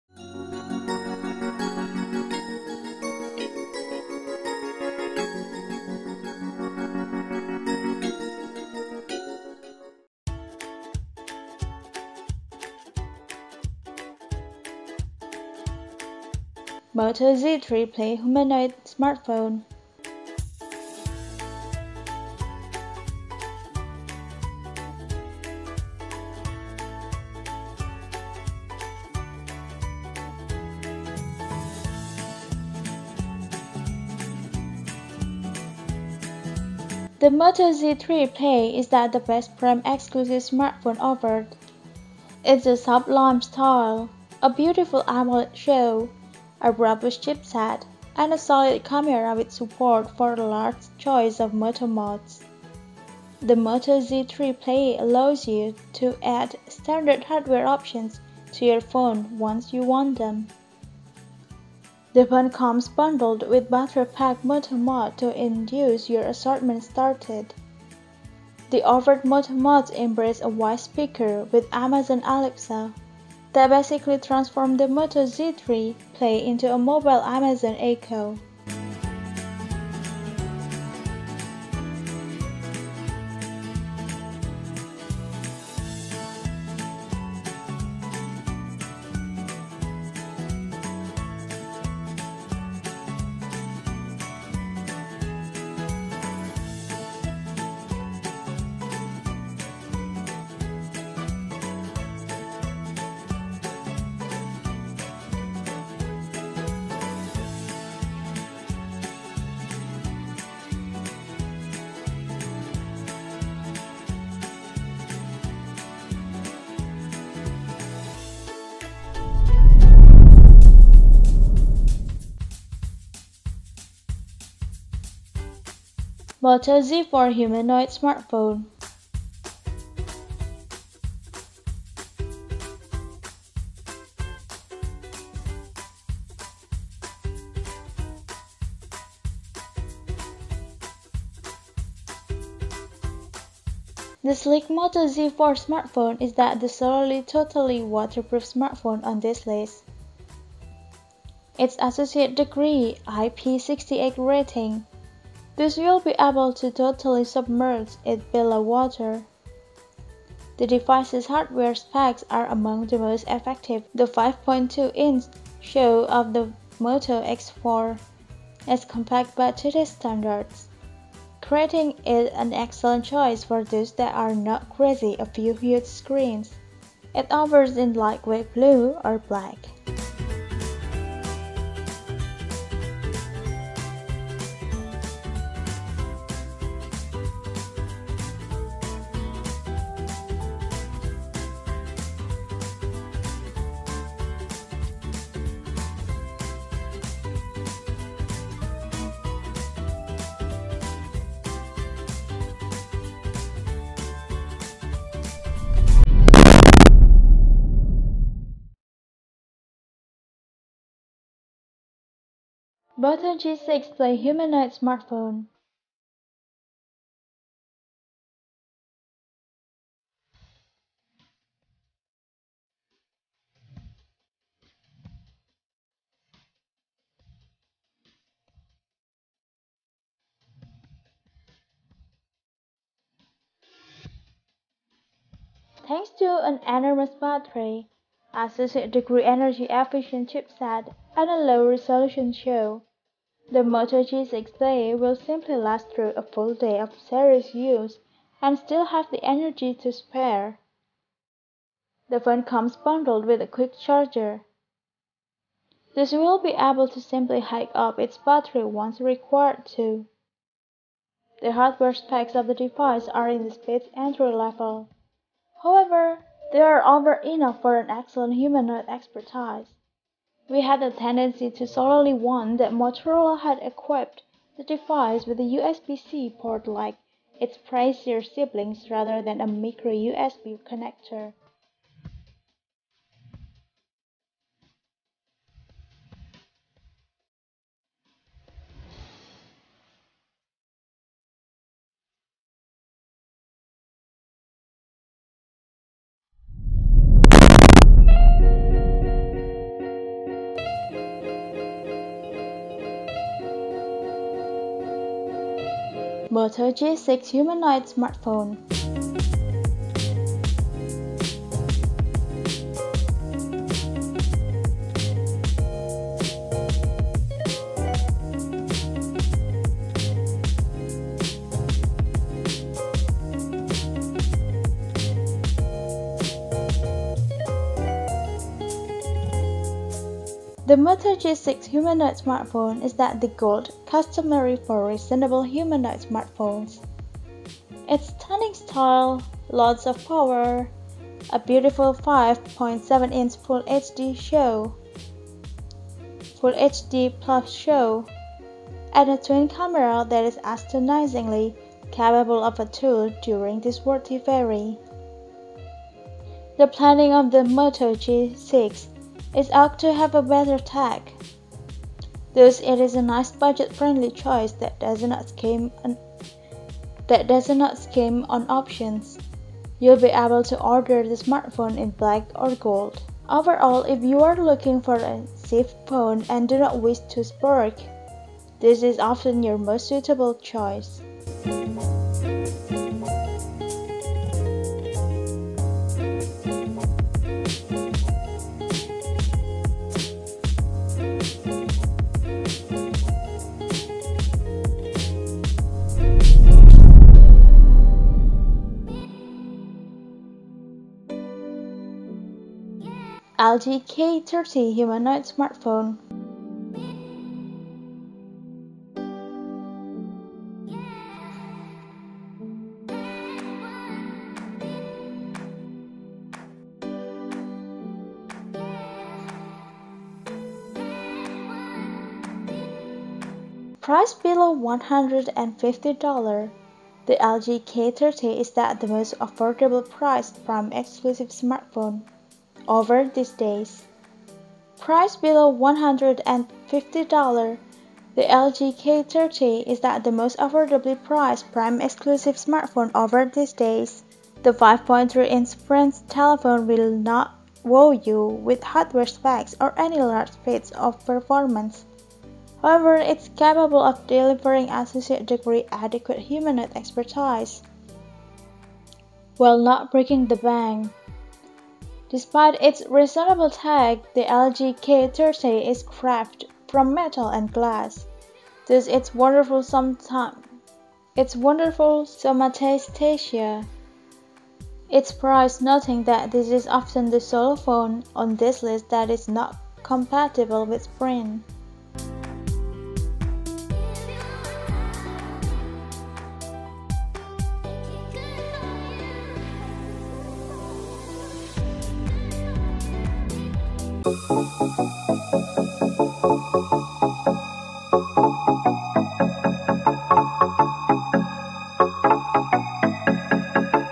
Amazon Prime Exclusive phone You Can Buy
Moto Z3 Play humanoid Smartphone
The Moto Z3 Play is that the best Prime Exclusive smartphone offered. it's a sublime style, a beautiful AMOLED show, a robust chipset, and a solid camera. With support for a large choice of Moto Mods, the Moto Z3 Play allows you to add standard hardware options to your phone once you want them. The phone comes bundled with battery pack Moto Mod to induce your assortment started. The offered Moto Mods embrace a wise speaker with Amazon Alexa, that basically transforms the Moto Z3 Play into a mobile Amazon Echo.

Moto X4 humanoid Smartphone
The sleek Moto X4 smartphone is that the solely totally waterproof smartphone on this list — it's associate degree IP68 rating, thus you'll be able to totally submerge it below water. The device's hardware specs are among the most effective in its worth vary. The 5.2-inch show of the Moto X4 is compact by today's standards, creating it an excellent choice for those that are not crazy a few huge screens. It's offered in lightweight blue or black.

Moto G6 Play humanoid Smartphone
Thanks to an enormous battery, associate degree energy-efficient chipset, and a low-resolution show, the Moto G6 Play will simply last through a full day of serious use and still have the energy to spare. The phone comes bundled with a quick charger, thus you'll be able to simply high up its battery once required, too. The hardware specs of the device are in spades entry-level, however, they are over enough for an excellent humanoid expertise. we have a tendency to solely want that Motorola had equipped the device with a USB-C port like its pricier siblings rather than a micro USB connector.

Moto G6 humanoid Smartphone
The Moto G6 humanoid smartphone is that the gold customary for reasonable humanoid smartphones. it's stunning style, lots of power, a beautiful five.7-inch Full HD+ show, and a twin camera that is astonishingly capable of a tool during this worth vary. the planning of the Moto G6 is ought to have a better tag, thus it is a nice budget-friendly choice that does not skimp on options. you'll be able to order the smartphone in black or gold. Overall, if you are looking for a chief phone and do not wish to splurge, this is often your most suitable choice.

LG K30 humanoid Smartphone
Priced below, $150, the LG K30 is that the most affordably-priced Prime Exclusive smartphone offered these days. The 5.3-inch French telephone will not wow you with hardware specs or any large feats of performance, however, it's capable of delivering associate degree adequate humanoid expertise while not breaking the bank. Despite its reasonable tag, the LG K30 is crafted from metal and glass, thus it's wonderful somatesthesia. It's price noting that this is often the sole phone on this list that is not compatible with Sprint.